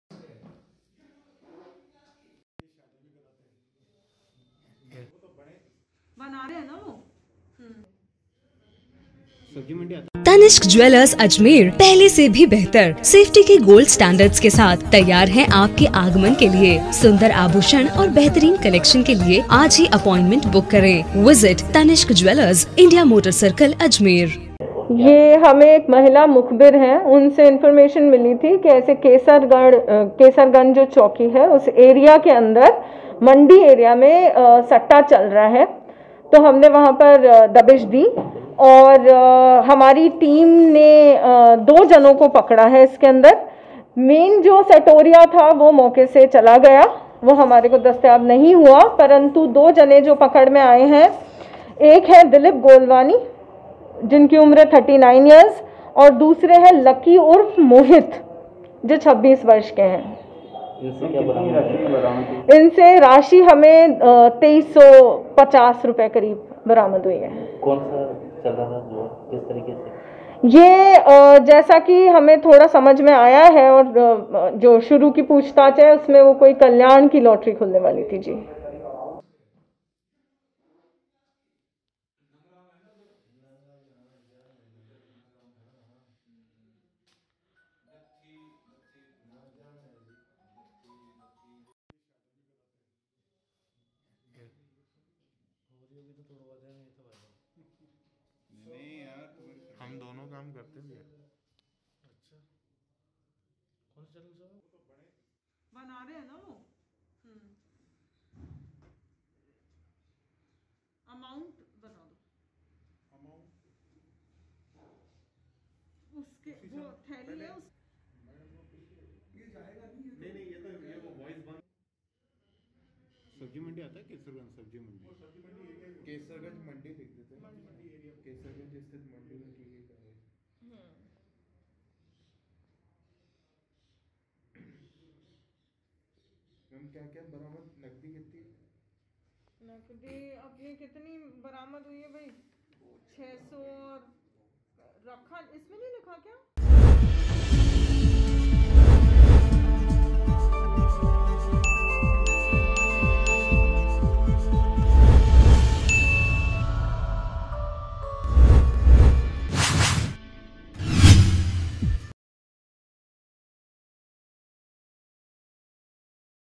Ajmer | क्लॉक टावर थाना पुलिस की बड़ी कार्यवाही | MTTV INDIA
क्लॉक टावर थाना पुलिस की बड़ी कार्यवाही
केसर गंज एरिया में सट्टे की सूचना पर पुलिस ने मौके से दो लोगों को दबोचा